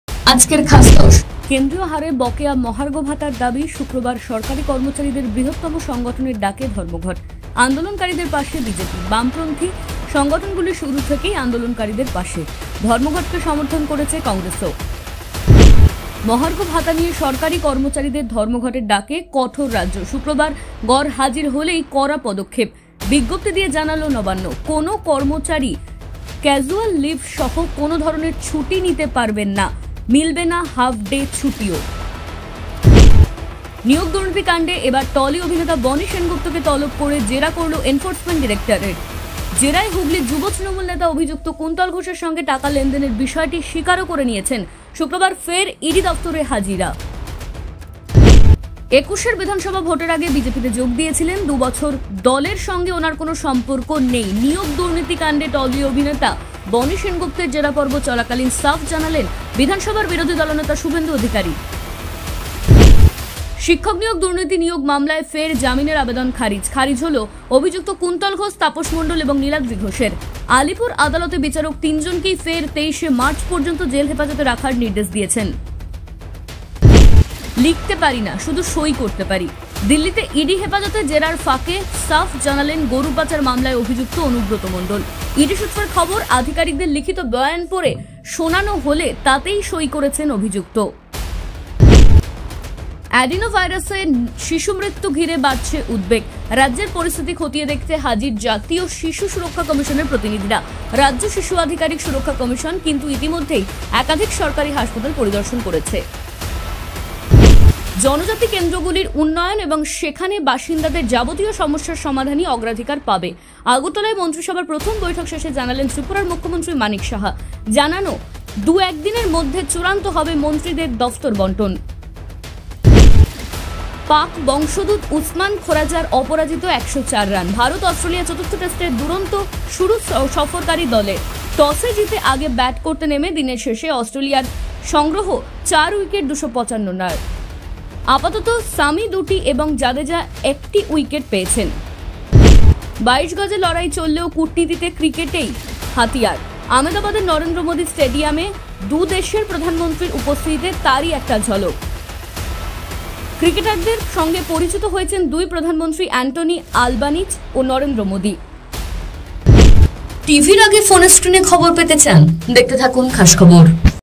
আজকের খাস দশ Today Bengali News Headlines। Top Bengali Headlines Today। Khas Khobor।
১) কেন্দ্রীয় হারে  মহার্ঘ ভাতার  দাবিতে শুক্রবার  ধর্মঘটের ডাক

কেন্দ্রীয় হারে বকেয়া মহার্ঘ ভাতার দাবি৷ শুক্রবার সরকারি কর্মচারীদের বৃহত্তম সংগঠনের ডাকে ধর্মঘট৷ আন্দোলনকারীদের পাশে বিজেপি। বামপন্থী সংগঠনগুলি শুরু থেকেই আন্দোলনকারীদের পাশে। ধর্মঘটকে সমর্থন করছে কংগ্রেসও৷

২) ধর্মঘট রুখতে   কড়া পদক্ষেপ     বিজ্ঞপ্তি দিয়ে      জানাল নবান্ন

মহার্ঘ ভাতা নিয়ে সরকারি কর্মচারীদের ধর্মঘটের ডাকে কঠোর রাজ্য৷ শুক্রবার গরহাজির হলেই কড়া পদক্ষেপ৷ বিজ্ঞপ্তি দিয়ে জানাল নবান্ন৷ কোনও কর্মচারী ক্যজুয়াল লিভ সহ কোনও ধরনের ছুটি নিতে পারবেন না, মিলবে না হাফ ডে ছুটিও৷

৩)  টলি অভেনেতা  বনি সেনগুপ্তকে  নিয়োগ দুর্নীতিতে   ইডির জেরা

নিয়োগ দুর্নীতিকাণ্ডে এবার টলি অভিনেতা বনি সেনগুপ্তকে তলব করে জেরা করল এনফোর্স ডিরেক্টরেট৷ জেরায় হুগলির যুব তৃণমূল নেতা অভিযুক্ত কুন্তল ঘোষের সঙ্গে টাকা লেনদেনের বিষয়টি স্বীকারও করে নিয়েছেন৷ শুক্রবার ফের ইডি দফতরে হাজিরা৷

৪) দলের সঙ্গে  সম্পর্ক নেই   বনিকাণ্ডে বিবৃতি   শুভেন্দু অধিকারীর

একুশের বিধানসভা ভোটের আগে বিজেপিতে যোগ দিয়েছিলেন৷ ২ বছর দলের সঙ্গে উনার কোনও সম্পর্ক নেই’৷ নিয়োগ দুর্নীতি কাণ্ডে টলি অভেনেতা বনি সেনগুপ্তের জেরাপর্ব চলাকালীন সাফা জানালেন বিধানসভার বিরোধী দলনেতা শুভেন্দু অধিকারী৷

৫) ফের ১৪ দিনের   জেল হেফাজত   কুন্তল সহ    ৩ অভিযুক্তের

শিক্ষক নিয়োগ দুর্নীতি নিয়োগ মামলায় ফের জামিনের আবেদন খারিজ৷ খারিজ হল অভিযুক্ত কুন্তল ঘোষ, তাপস মণ্ডল এবং নীলাদ্রি ঘোষের৷ আলিপুর আদালতের বিচারক, তিনজনকেই ফের ২৩ মার্চ পর্যন্ত জেল হেফাজতে রাখার নির্দেশ দিয়েছেন৷

৬) ইডির জেরায়   ফ্যাসাদে অনুব্রত    লিখিত বয়ানে   সই অভিযুক্তের

লিখতে পারি না, শুধু সই করতে পারি৷ দিল্লিতে ইডি হেফাজতে জেরার ফাঁকে সাফ জানালেন গরু পাচার মামলায় অভিযুক্ত অনুব্রত মণ্ডল৷ ইডি সূত্রে খবর, আধিকারিকদের লিখিত বয়ান পড়ে শোনান হলে তাতেই সই করেছেন অভিযুক্ত৷

৭) অ্যডিনো আতঙ্কে   বাড়ছে উদ্বেগ  পরিস্থিতি দেখতে  রাজ্যে কমিশন

অ্যাডিনো ভাইরাসে ন শিশুমৃত্যু ঘিরে বাড়ছে উদ্বেগ৷ রাজ্যের পরিস্থিতি খতিয়ে দেখতে হাজির জাতীয় শিশু সুরক্ষা কমিশনের প্রতিনিধিরা৷  রাজ্য শিশু অধিকার সুরক্ষা কমিশন কিন্তু ইতিমধ্যেই একাধিক সরকারি হাসপাতাল পরিদর্শন করেছে৷

৮) জনজাতিদের সমস্যা   সমাধানে অগ্রাধিকার    মন্ত্রিসভার বৈঠকে  জানান মানিক

জনজাতি কেন্দ্রগুলির উন্নয়ন এবং সেখান বাসিন্দাদের যাবতীয় সমস্যার সমাধানই অগ্রাধিকার পাবে৷ আগরতলায় মন্ত্রিসভার প্রথম বৈঠক শেষে জানালেন, ত্রিপুরার মুখ্যমন্ত্রী মানিক সাহা৷  জানান, দু-একদিনের মধ্যে চূড়ান্ত হবে মন্ত্রীদের দফতর বন্টন৷

৯) খোয়াজার ১০৪*     চতুর্থ টেস্টে   মজবুত ভিতে   সফরকারী দল

পাক বংশোদ্ভূত উসমান খোয়াজার অপরাজিত ১০৪ রান৷ ভারত-অস্ট্রেলিয়ার চতুর্থ টেস্টে দুরন্ত শুরু সফরকারী দলের৷ টসে জিতে আগে ব্যাট করতে নেমে  দিনের শেষে  অস্ট্রেলিয়ার সংগ্রঙ ৪ উইকেটে ২৫৫ রান৷ আপাতত  শামি দুটি এবং জাদেজা ১টি উইকেট পেয়েছেন

১০)  কূটনীতিতে ক্রিকেট   ক্রিকেটারদের সঙ্গে  পরিচিত হলেন   ২ প্রধানমন্ত্রী

২২ গজে লড়াই চললেও, কূটনীতিতে ক্রিকেটকেই হাতিয়ার৷ আমেদাবাদের নরেন্দ্র মোদী স্টেডিয়ামে দু দেশের প্রধানমন্ত্রীর উপস্থিতি তারই একটা ঝলক৷ ক্রিকেটারদের সঙ্গে পরিচিত হয়েছেন দুই প্রধানমন্ত্রী অ্যান্টনি আলবানিজ় ও  নরেন্দ্র মোদী৷

টিভির আগে ফোনের স্ক্রিনে খবর পেতে চান, দেখতে থাকুন খাস খবর